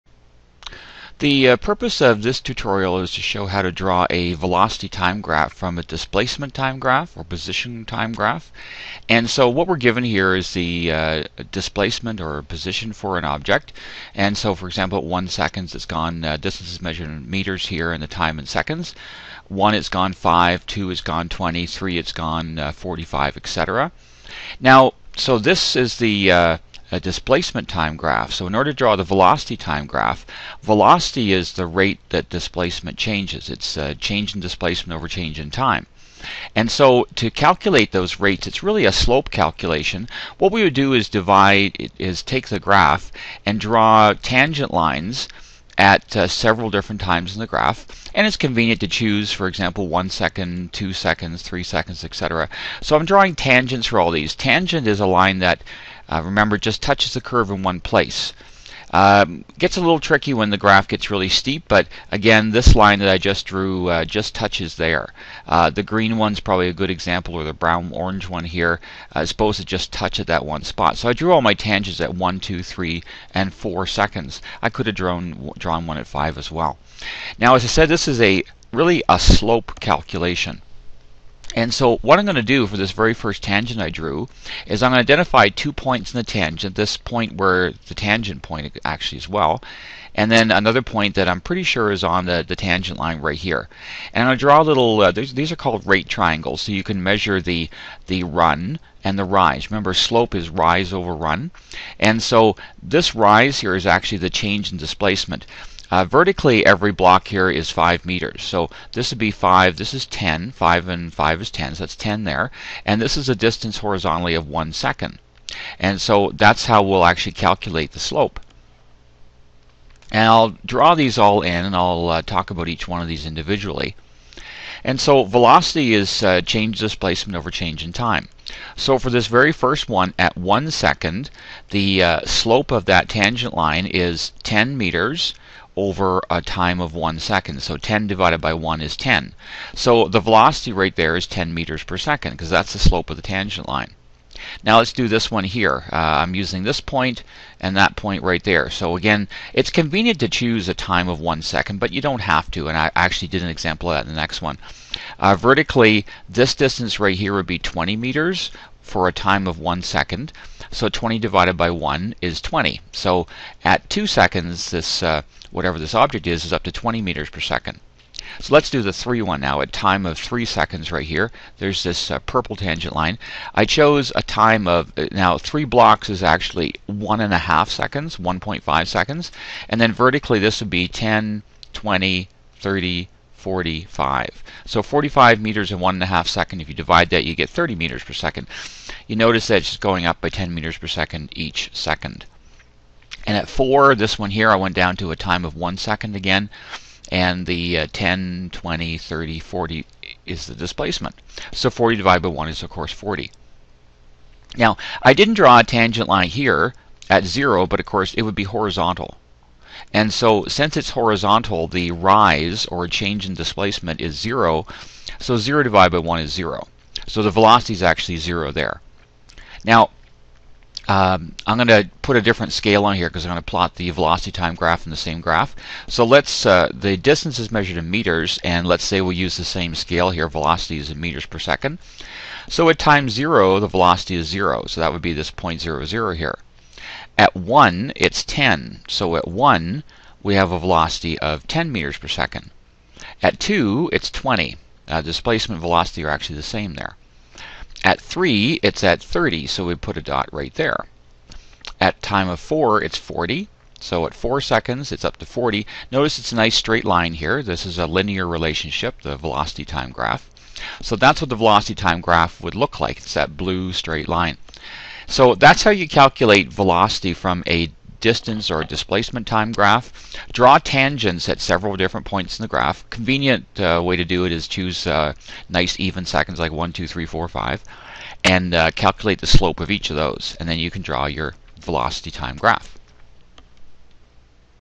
Drawing Velocity-Time Graphs
This video shows how to draw a velocity-time graph from a displacement time graph by using slopes of tangent lines.  This video was created for the SPH4C college physics course in the province of Ontario, Canada.